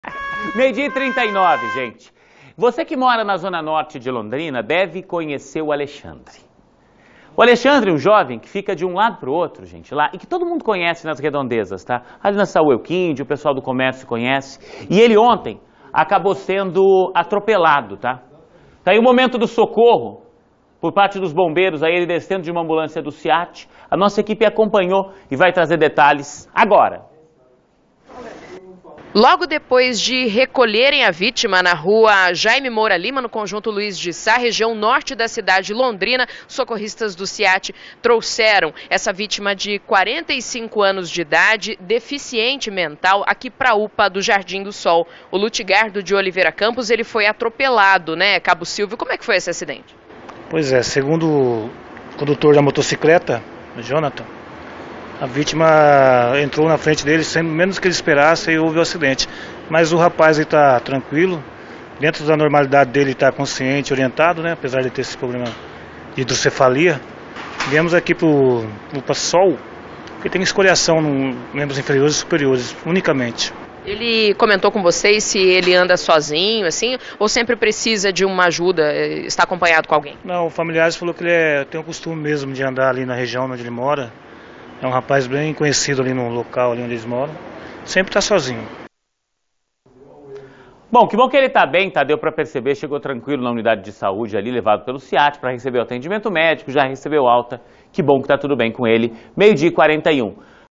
Deficiente é atropelado na zona norte
Um homem com problemas mentais foi atropelado na zona norte de Londrina, ele precisou de atendimento por parte do SIATE.